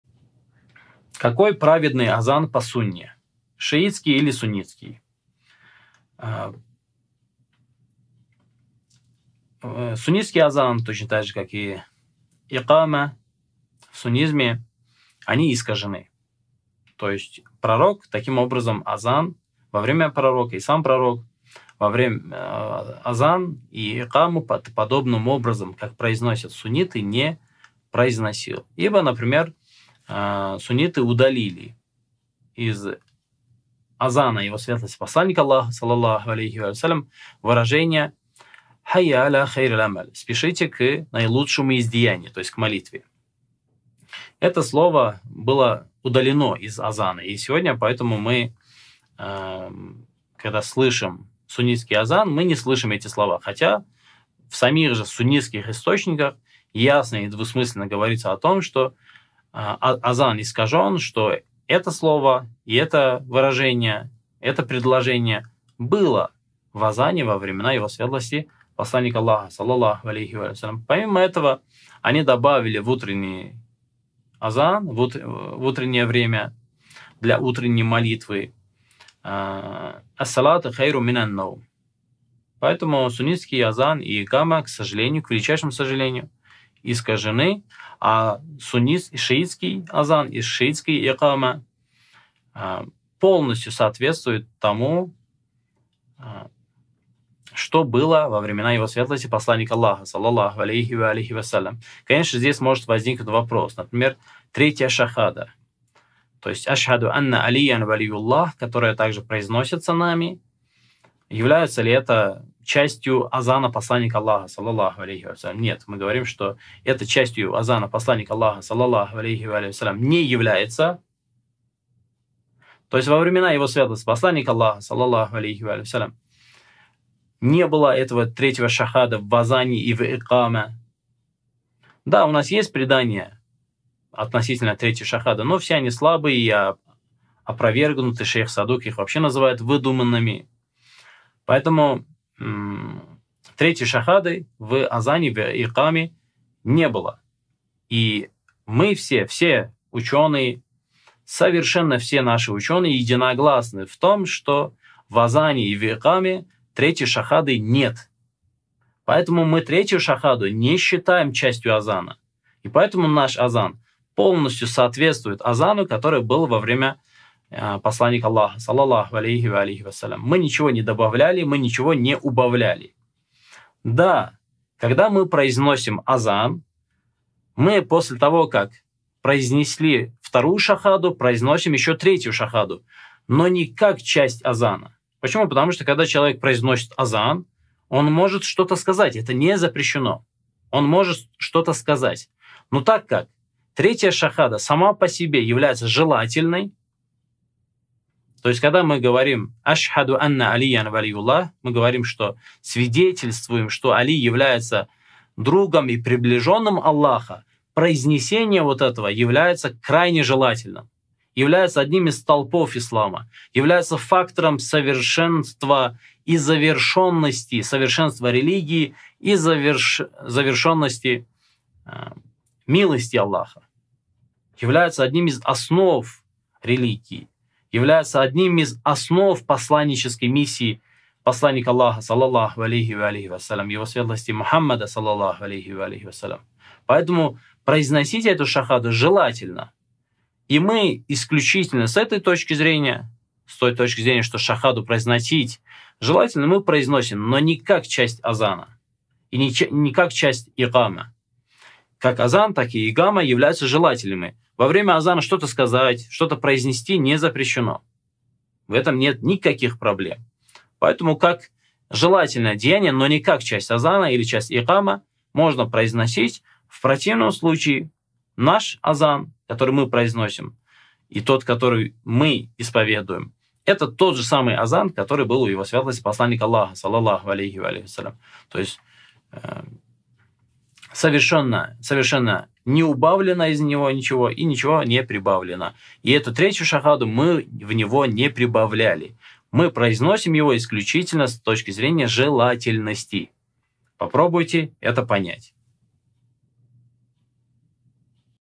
Какой азан соответствует сунне, шиитский или суннитский?
Какой азан соответствует сунне, шиитский или суннитский? Курбан Мирзаханов